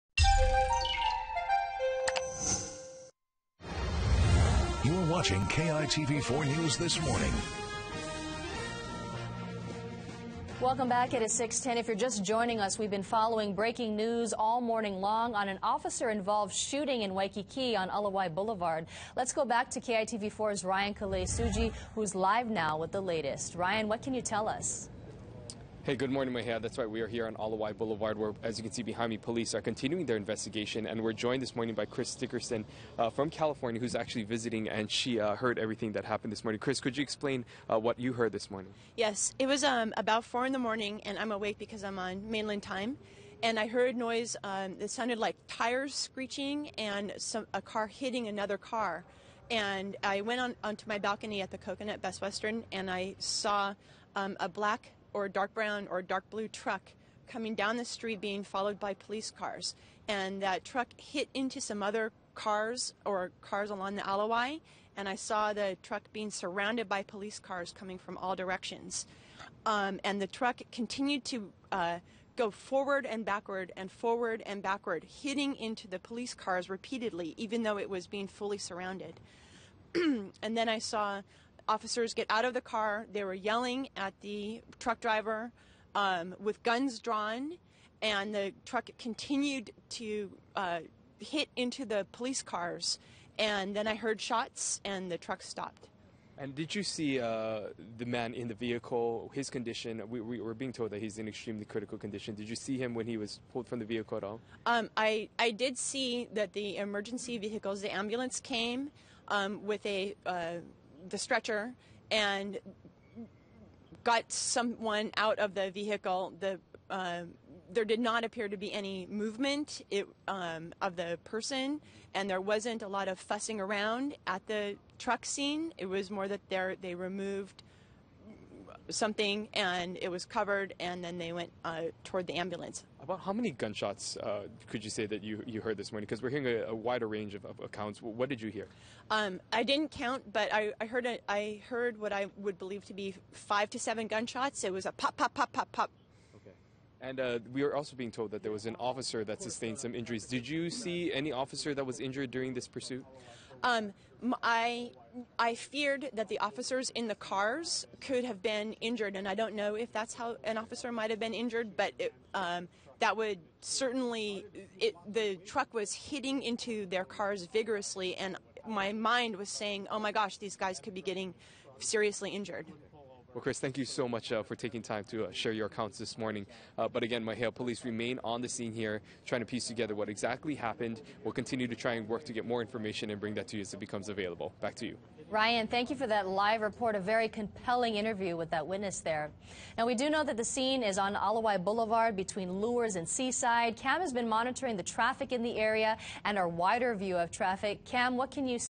Witness recounts scene at Waikiki shooitng
An officer-involved shooting in Waikiki Tuesday morning left one man in critical condition and a police officer in stable condition. KITV4's Ryan Kalei Tsuji interviewed a witness from the scene of the crime. For news you need to know,